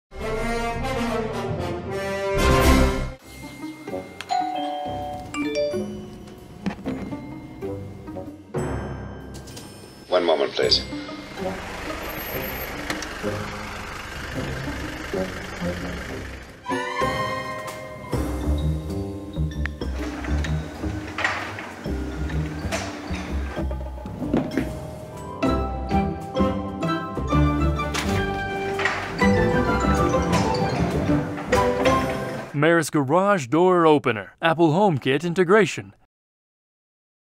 Meross Collie smart garage controller - Secure your door with world's top encryption
Meross Collie SmartGarageController
✔ Works with AppleHomeKit, GoogleAssistant, and Amazon
✔ Support up to over 200 brands and 1,600 models garage opener motor
✔ Secure your home with the world's top encryption
✔ Simple set up and upgrade your garage today